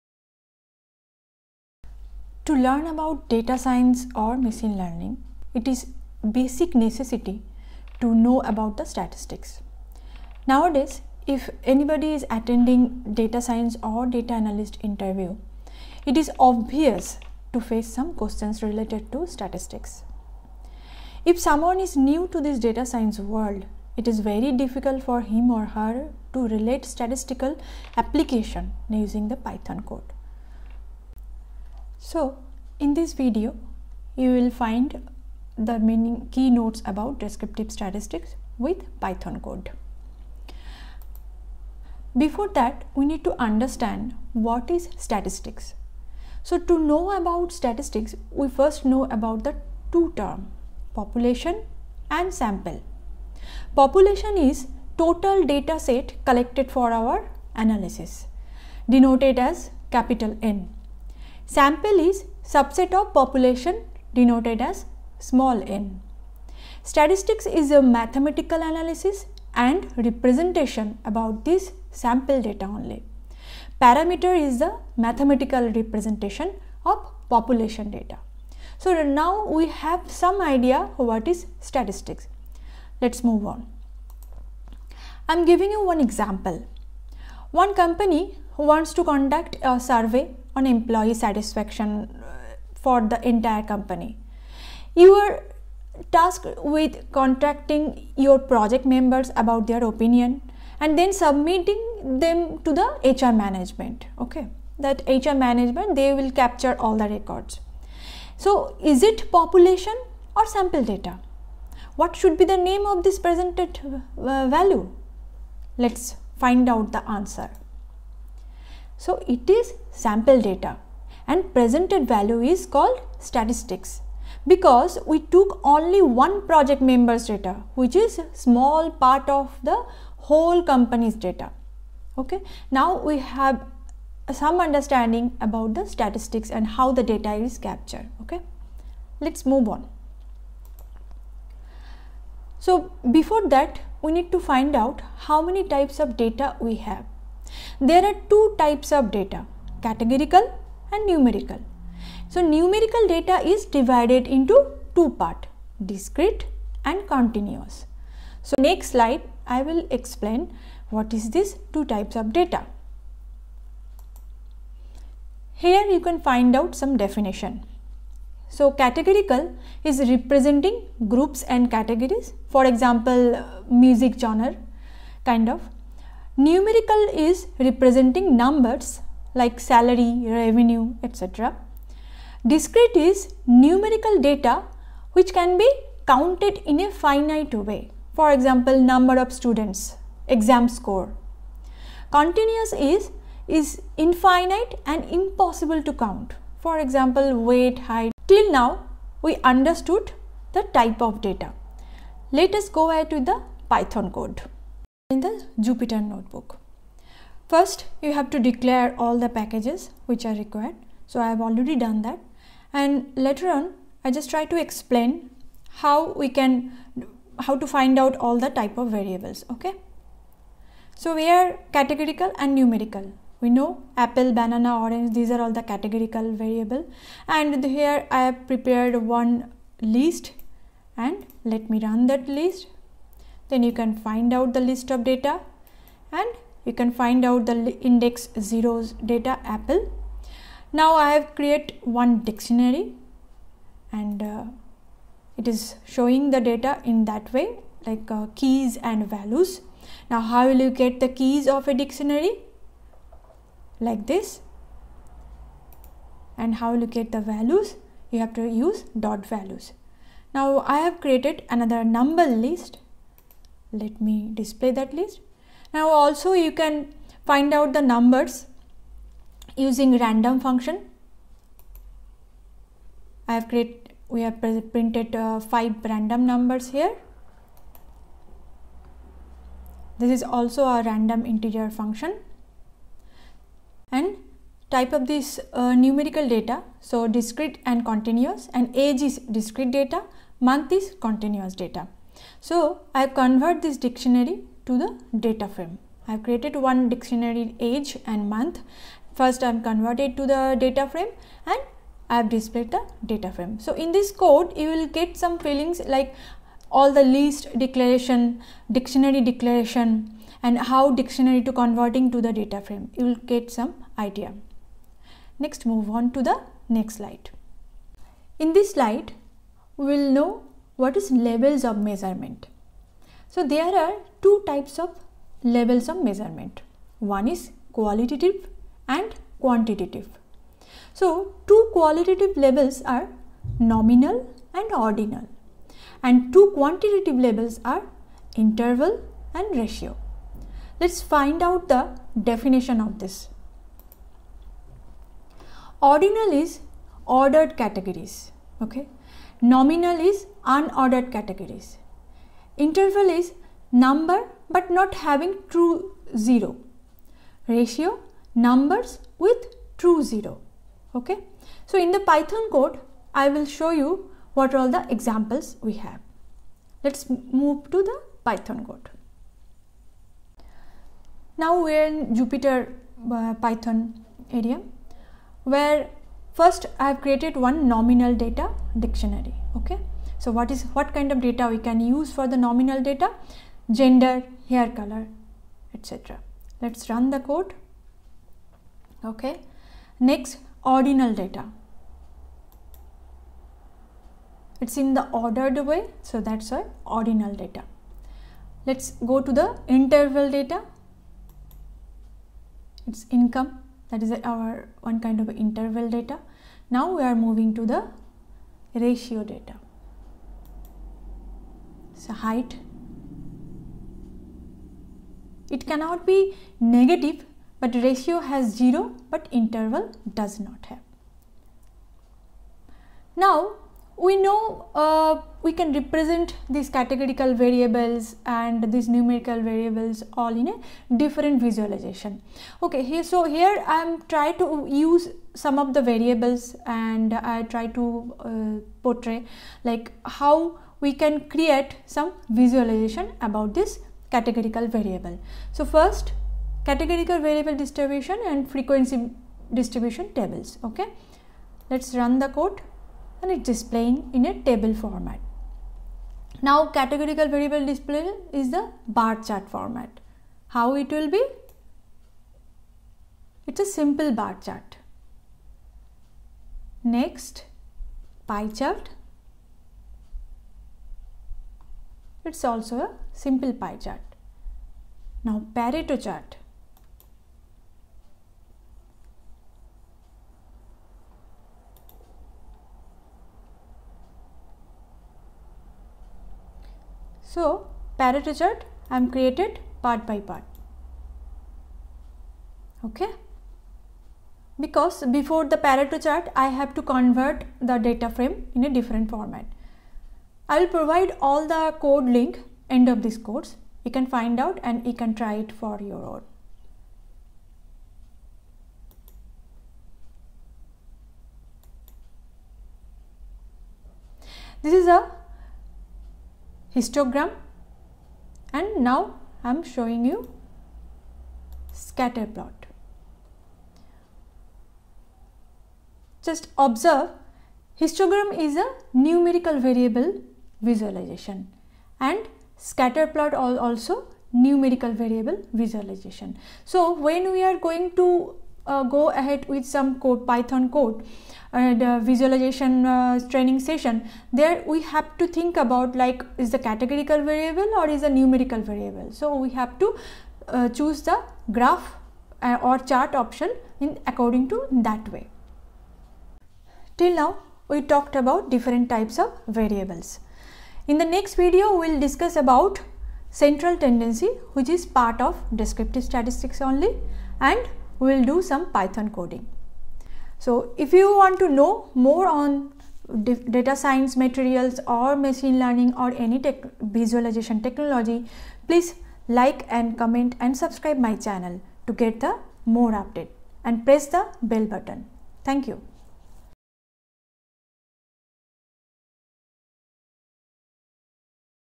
How to do Python coding for Descriptive Statistics | Explained with code sample
Descriptive Statistics with Python Part 1:
To learn about data science or machine learning, it is a basic necessity to know about statistics. Nowadays if anybody is attending data science or data analyst interviews, it is obvious to face some questions related to statistics. If someone is new to the data science world, it is very difficult for him/her to relate statistical applications using Python code.
Chapter Description:
0:00 Introduction
0:41 What is Statistics
1:24 Understanding Statistics with example
2:21 Types of Data
2:46 Definition of Types of Data
3:35 Python Coding
5:18 Levels of Measurement
6:10 Python Coding
9:17 Conclusion

This channel is created to prepare tutorial videos related to different technologies like Python , R language, Power BI, Tableau, SQL, Excel, Google Data Studio etc.